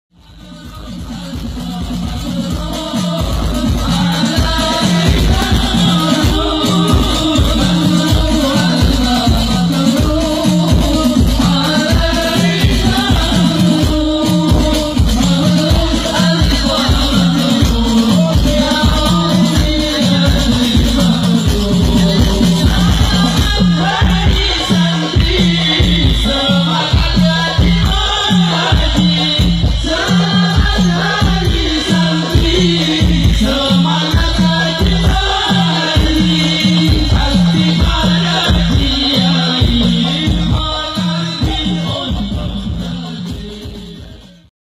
HADIRILAH PENGAJIAN UMUM DALAM RANGKA HAFLAH KHOTMIL QUR'AN KE-24 PPTQ ASNAWIYYAH
HADIRILAH PENGAJIAN UMUM DALAM RANGKA HAFLAH KHOTMIL QUR'AN KE-24 PPTQ ASNAWIYYAH PILANGWETAN KEBONAGUNG DEMAK BERSAMA HABIB UMAR MUTHOHAR DAN GANDRUNG NABI